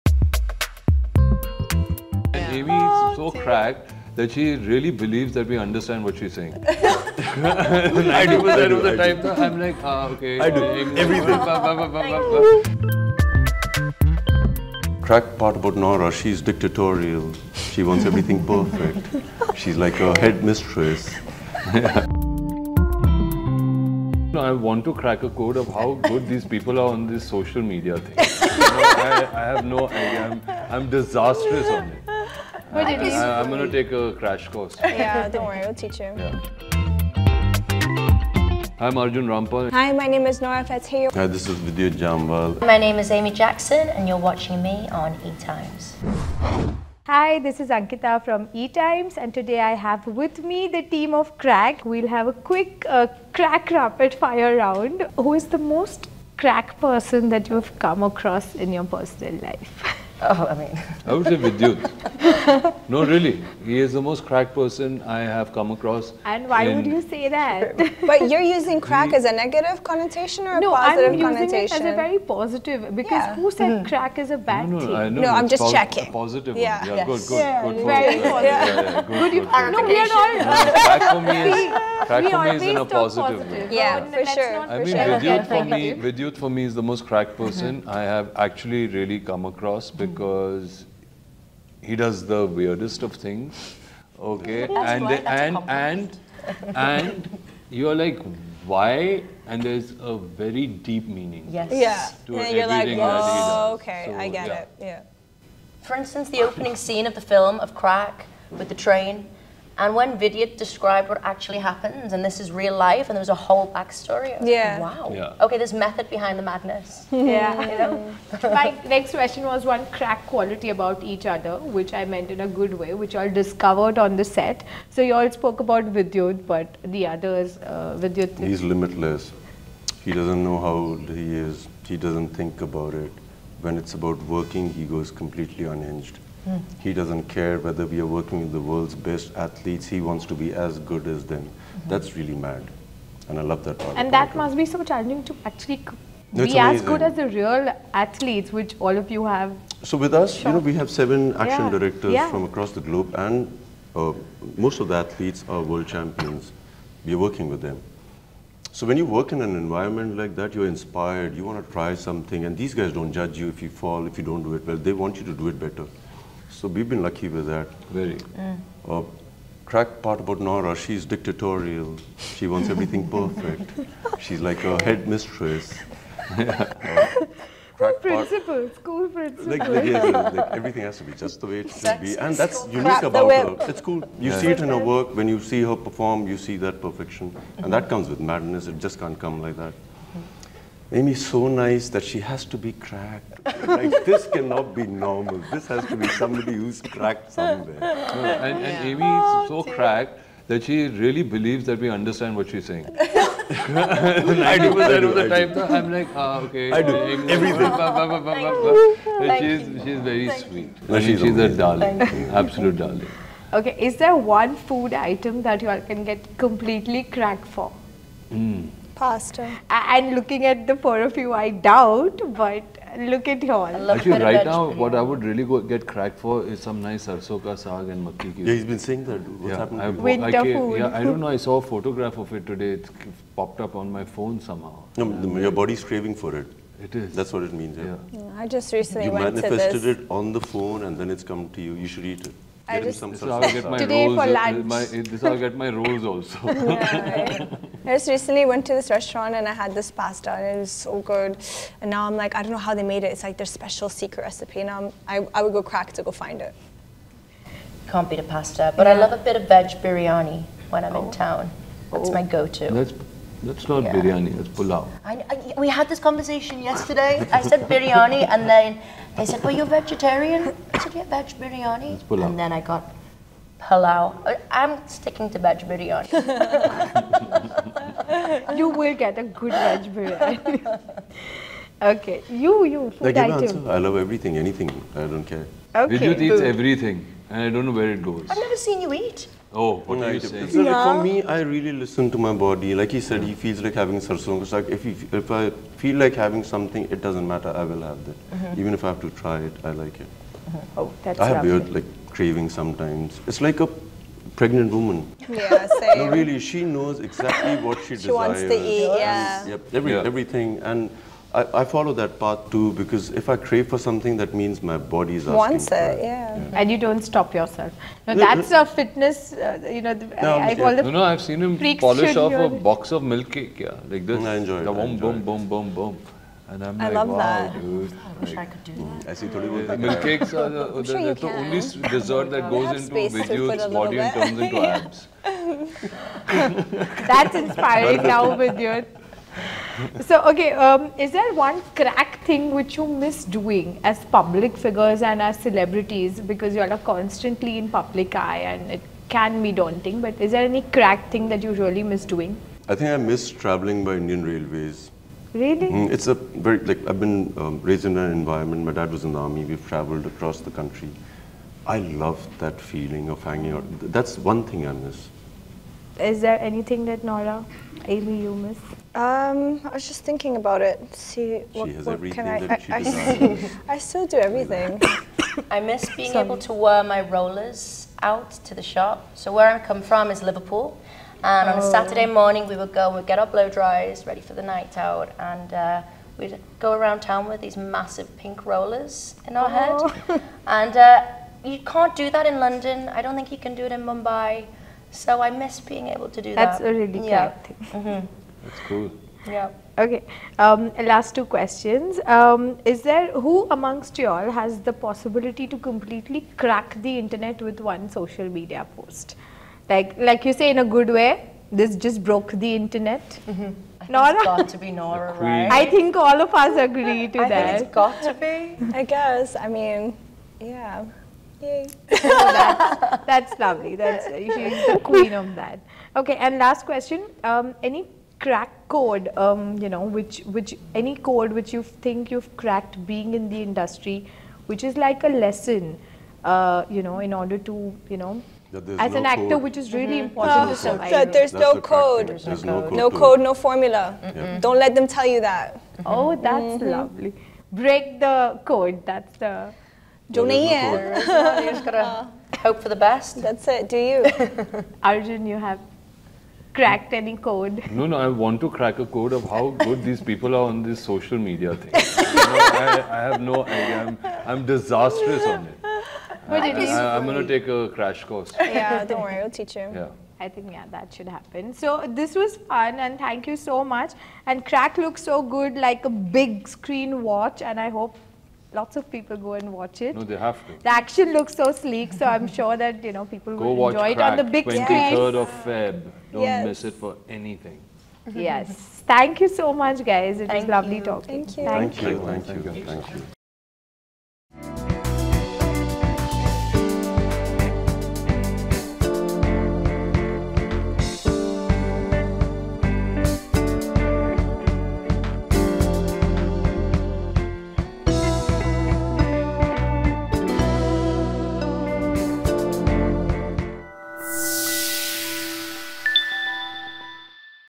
Crakk Rapid Fire Ft. Vidyut Jammwal, Nora Fatehi, Arjun Rampal & Amy Jackson
Join the Crakk team for an exclusive and entertaining chat with ETimes! Get ready to dive into their dynamic bond as Vidyut Jammwal, Nora Fatehi, Arjun Rampal & Amy Jackson spill the beans on who's the most 'crack' personality and who can break the internet with just one post. Don't miss out on their fun anecdotes—watch now for a dose of laughter and camaraderie!